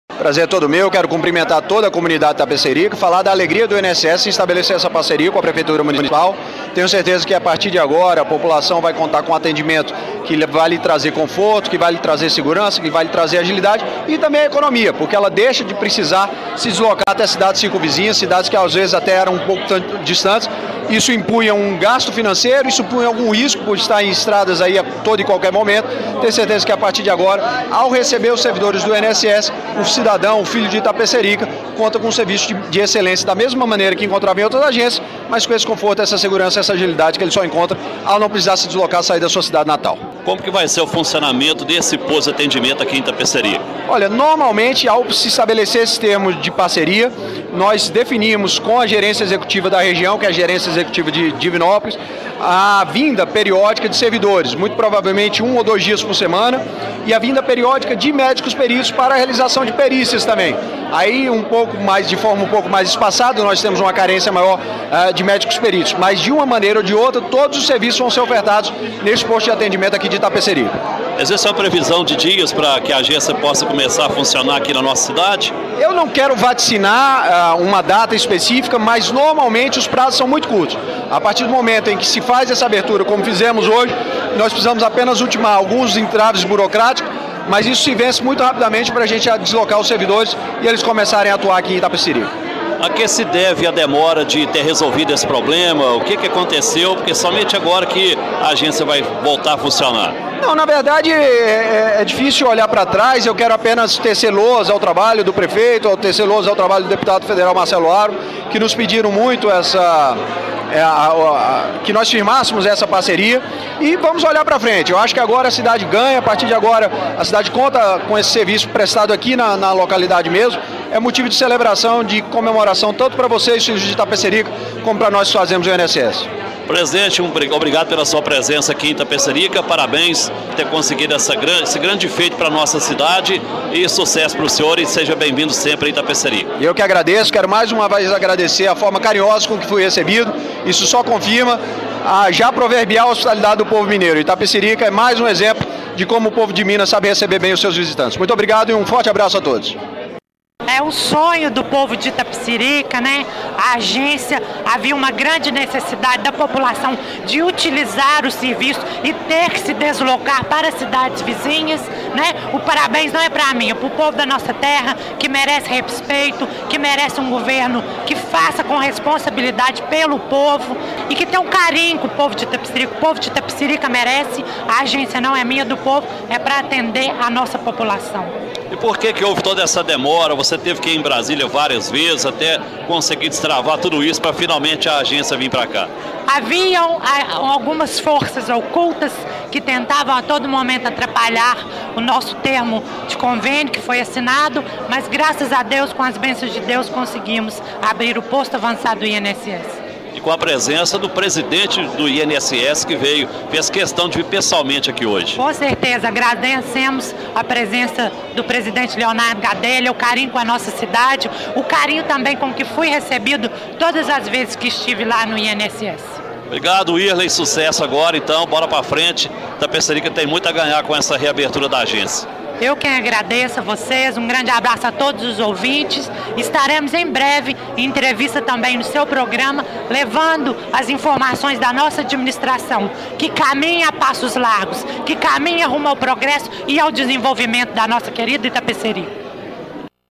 Entrevista Leonardo Gadelha Wirley Reis
Posto de atendimento do INSS é inaugurado em Itapecerica/MG